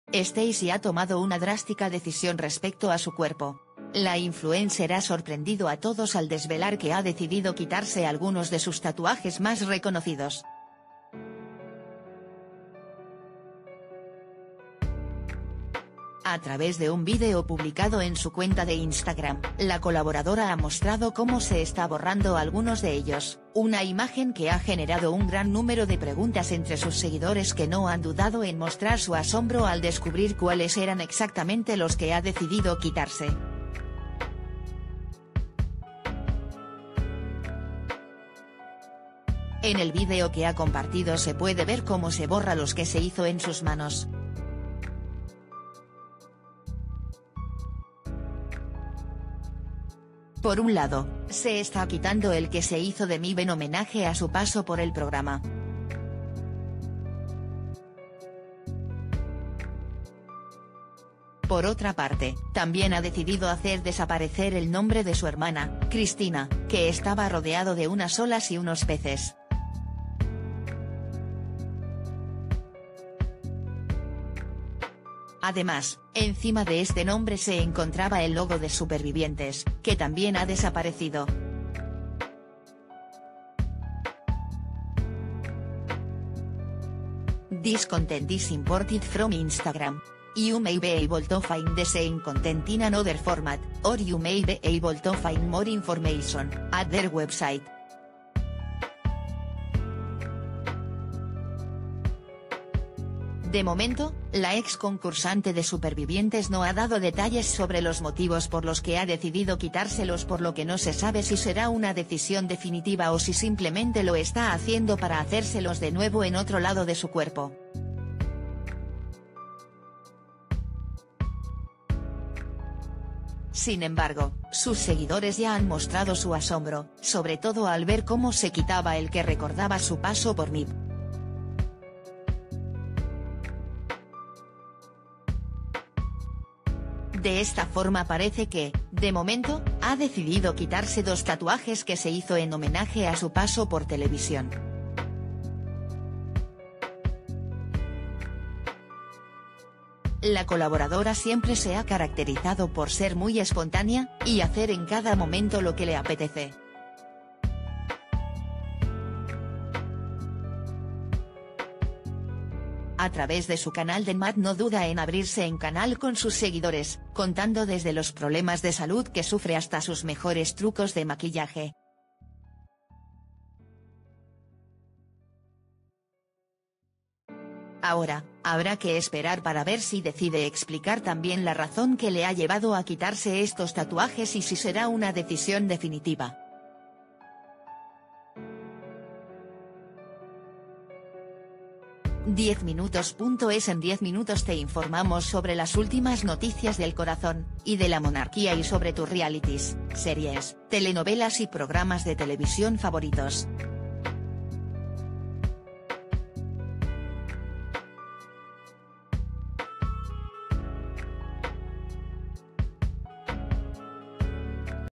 Steisy sorprende al quitarse algunos de sus tatuajes
Steisy ha tomado una drástica decisión respecto a su cuerpo. La 'influencer' ha sorprendido a todos al desvelar que ha decidido quitarse algunos de sus tatuajes más reconocidos. A través de un vídeo publicado en su cuenta de 'Instagram', la colaboradora ha mostrado cómo se está borrando algunos de ellos, una imagen que ha generado un gran número de preguntas entre sus seguidores que no han dudado en mostrar su asombro al descubrir cuáles eran exactamente los que ha decidido quitarse.En el vídeo